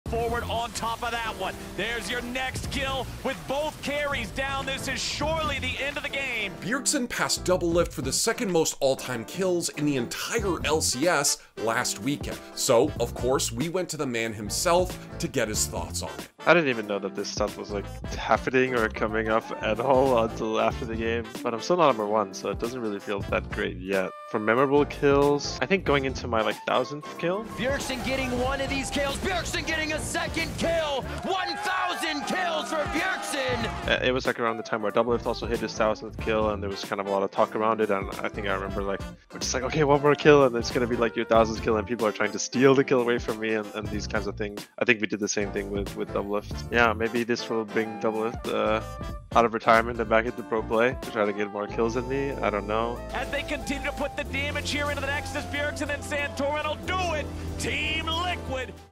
Bjergsen Beat Doublelift's LCS Kill Total
Bjergsen passed Doublelift for the second most all-time kills in the entire LCS last weekend.

So we went to the man himself to get his thoughts about it.

Footage courtesy of:
Lol Esports on YouTube